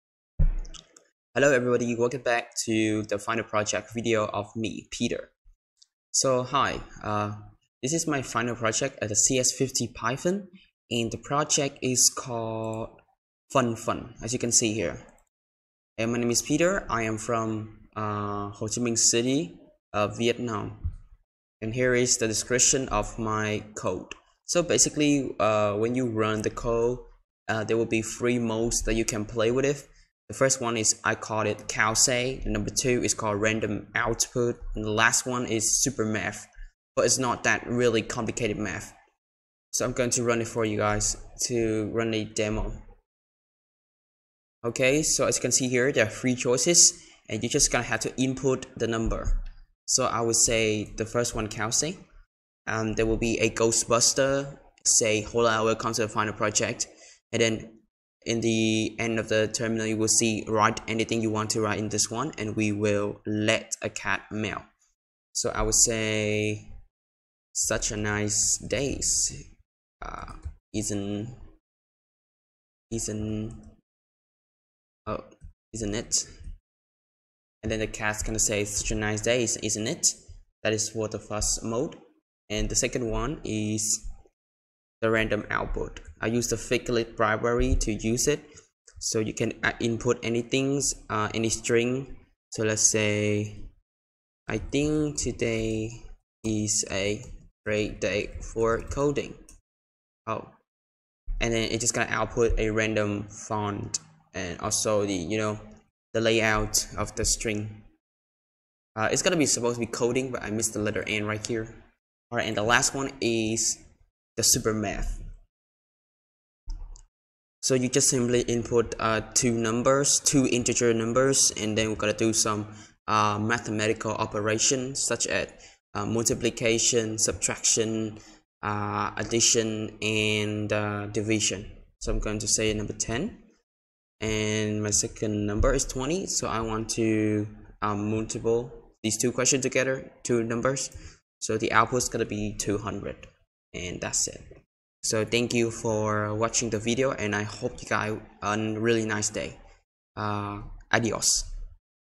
My CS50P Final Project
Hi, this video is a demo for Peter's final project of CS50 Python's course. It’s called “FunFun”, I cannot explain how I got this name up but as the matter of fact it’s right here. Technically, it has 3 major functions that you can use. Font customization, output style and maths game (it’s not a really game, work similarly like a hand calculator). However, it does meet the standard of CS50’s requirements. Eventually, I did earn a free certificate from CS50P :). It’s a really interesting journey for beginners like myself when stepping into Computer Science major.

I planned to make a virtual assistant for the final project, however I encountered few issues with the workspace so I couldn’t use it for this final project. Instead, I made a simple code for the final project.

But I promise everyone that I will upload the virtual assistant project (I made it pretty basic comes with multiple libraries but it’s fun to play with)